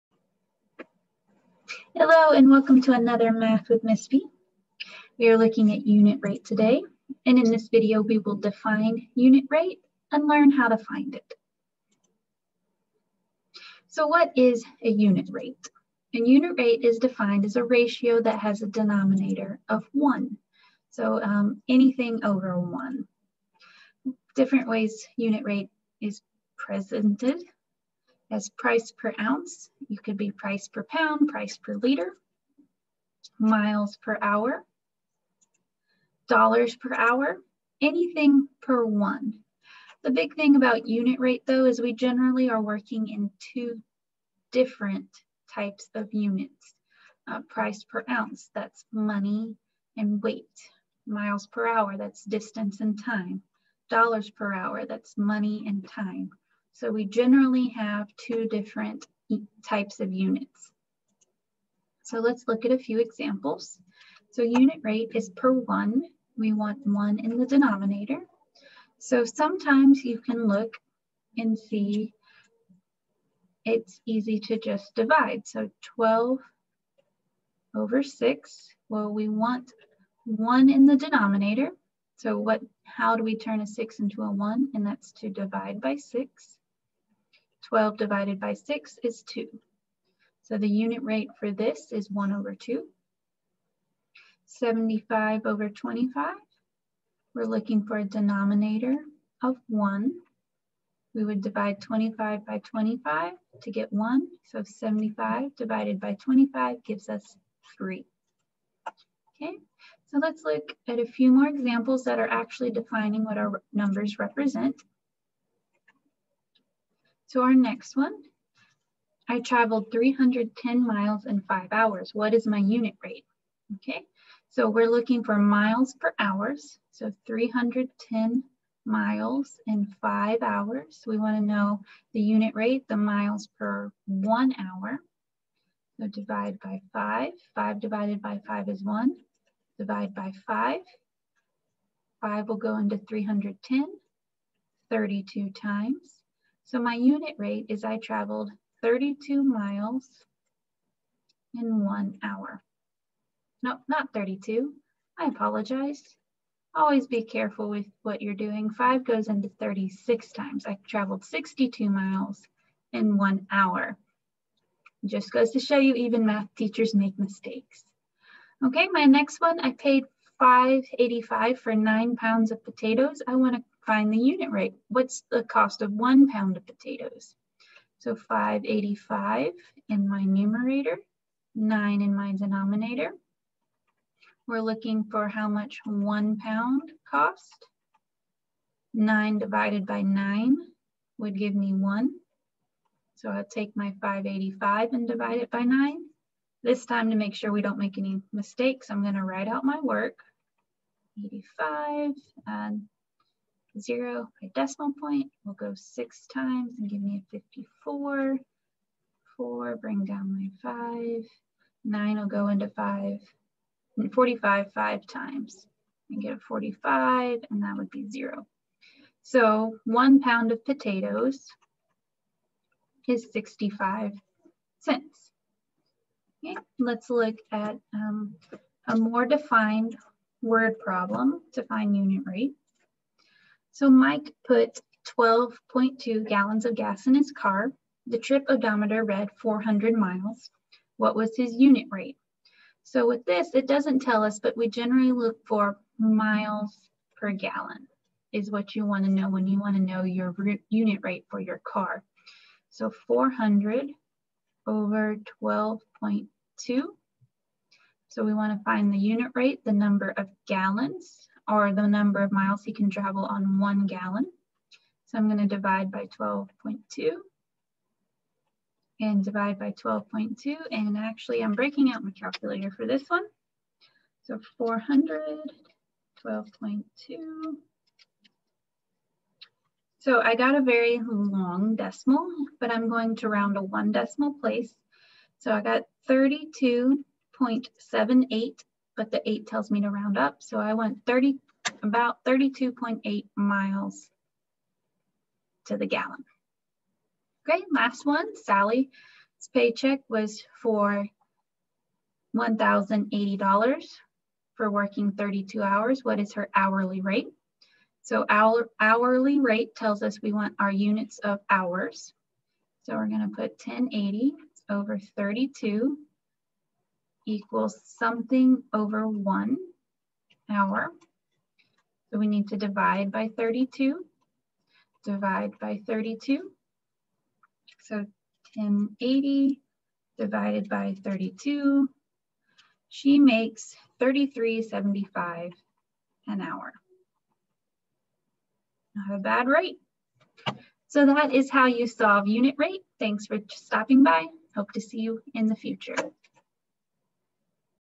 Finding Unit Rate | Math with Mrs. V
In this video, we will look at how to find the unit rate.

MGSE7.RP.1